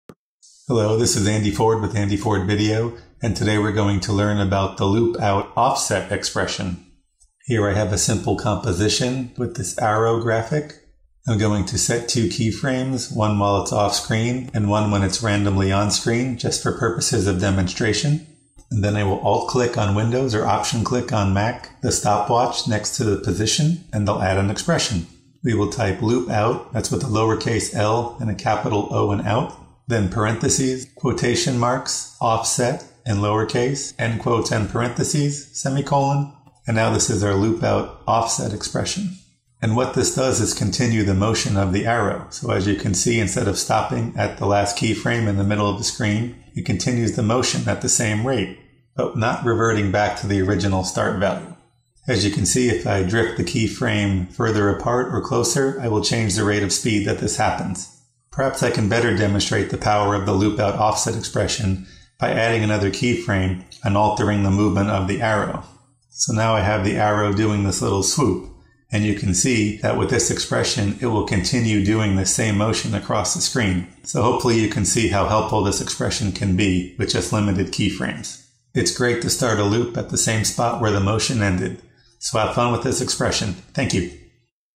After Effects Tutorial - loopOut Offset Expression
The loopOut Offset expression is an underrated After Effects expression that is useful to help continue motion after a keyframe. It can help you to start a loop at the same spot where your keyframed motion ended. This short tutorial video will explain how to implement this expression in your workflow.

loopOut(“offset”);